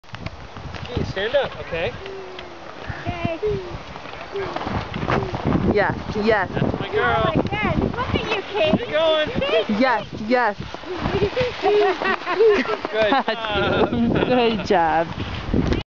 Cate at Breckenridge March 24
Mostly she skied with us, but on the flat areas, she did it herself!